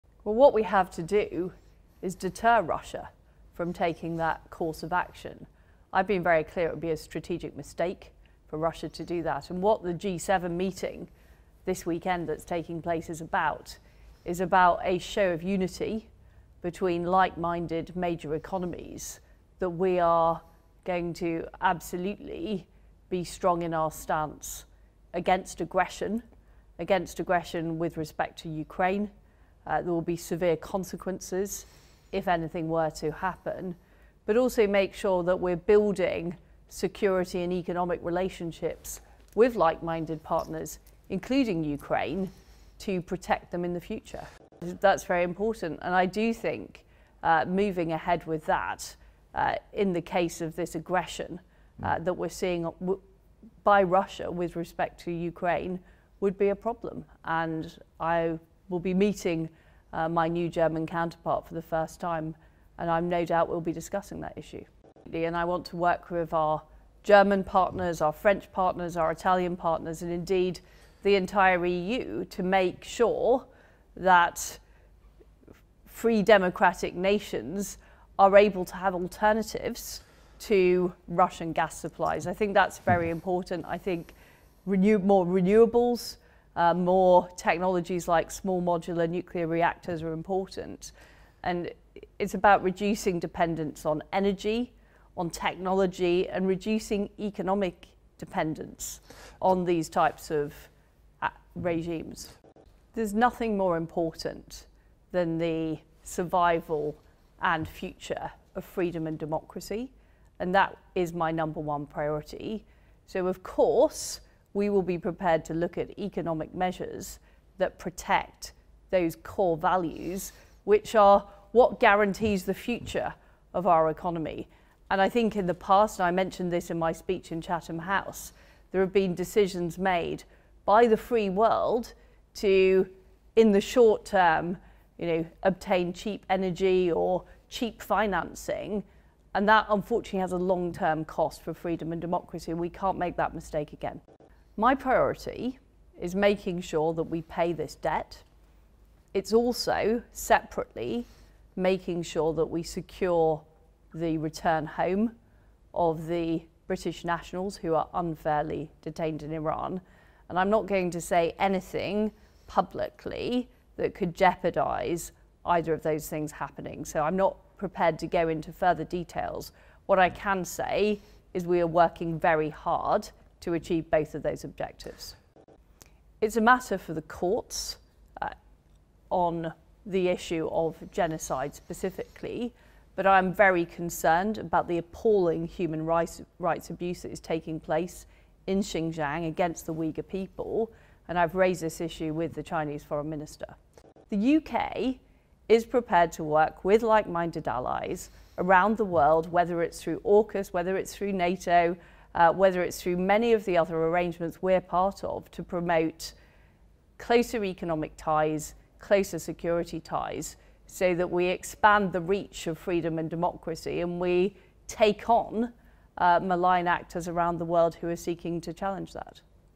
Britain says Russian attack on Ukraine would be a 'grave mistake'
British Foreign Secretary Liz Truss warned Russia that it would be making a "grave mistake" if it attacked Ukraine and said London was working closely with its NATO allies to support Ukraine.